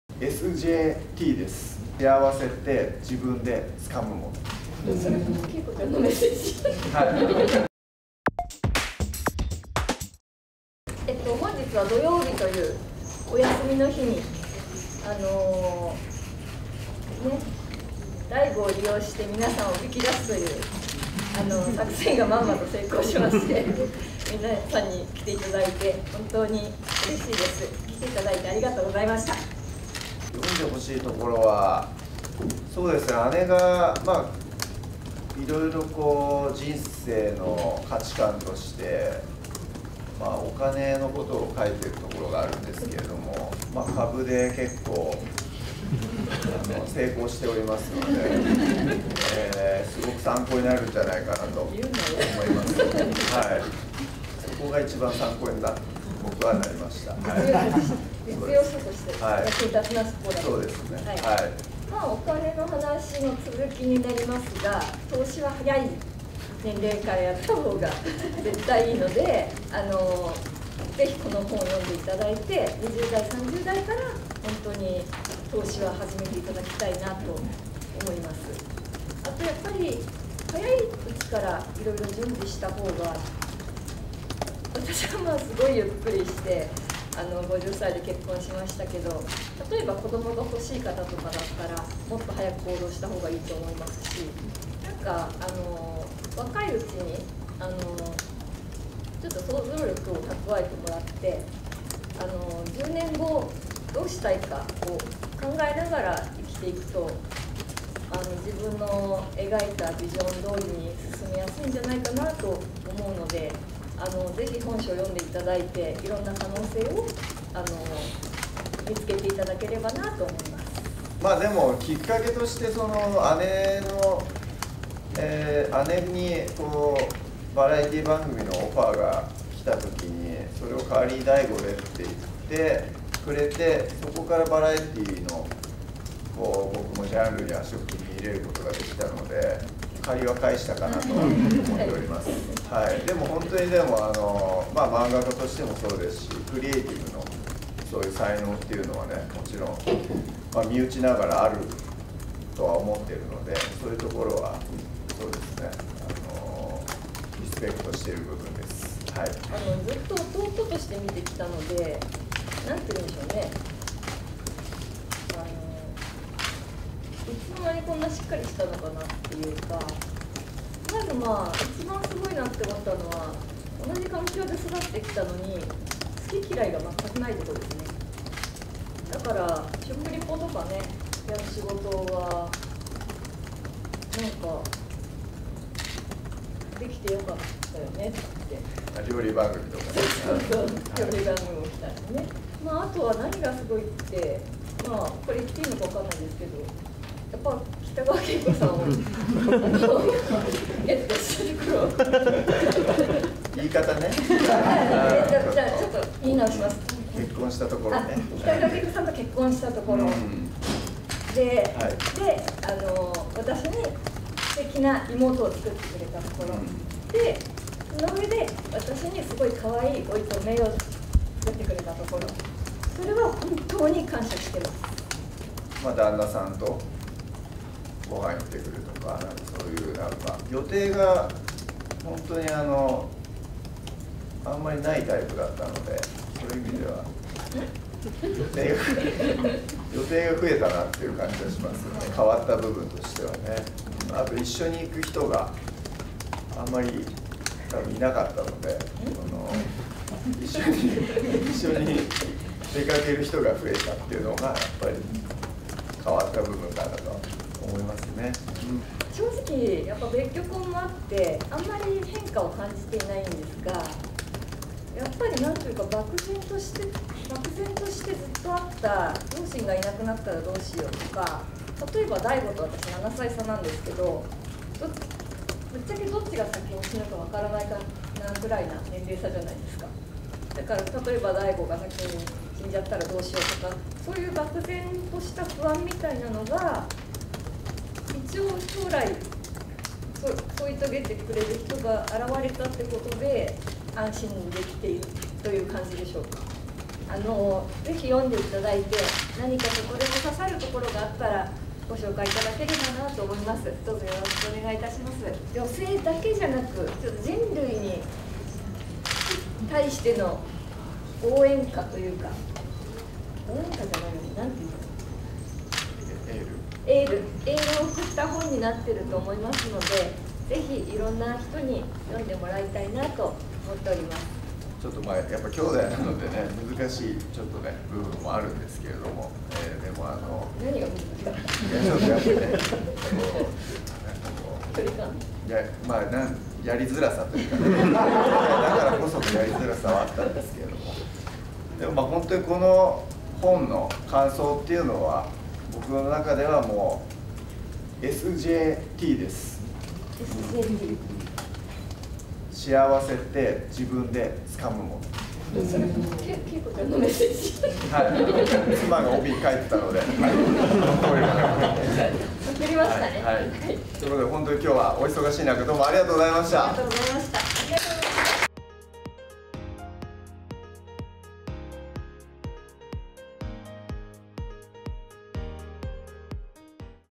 影木栄貴とDAIGOの姉弟トーク実現、結婚エッセイの感想は「SJY」
影木栄貴さんによる初のエッセイ「50婚 影木、おひとり様やめるってよ」の刊行を記念し、5月25日に東京・アニメイト池袋本店9階のanimate hall WHITEで記者会見が行われ、影木さんと弟のDAIGOさんが出席しました。

0:00〜 挨拶
0:34〜 エッセイの中で特に読んでほしいところ
2:01〜 お互いに尊敬・感謝しているところ
3:47〜 影木栄貴が結婚して変わった部分、変わらない部分
4:58〜 最後にメッセージ

【写真＆レポートはこちら】
影木栄貴の結婚エッセイ刊行で姉弟トーク実現、DAIGOの感想「SJT」は妻の言葉を拝借